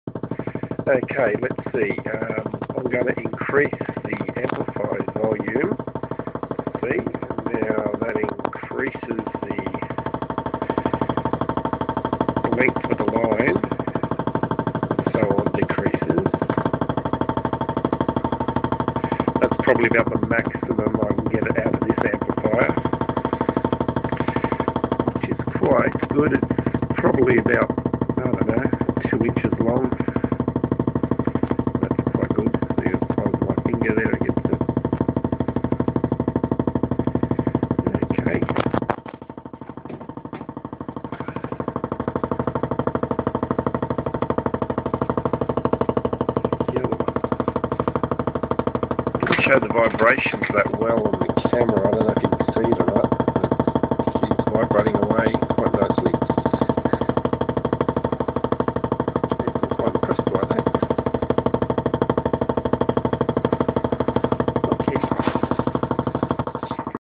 Voice coil harddrive line scanning adjustment
Vibrating mirror horizontal ine test 2 width control ,i would be using this line on the veritical for nbtv btw,these tests were a year or so back electronic movement of the mirror works much better than mechcanical switching .
The waveform is a sawtooth thats fed to a amp then to the old hard drive voice coil.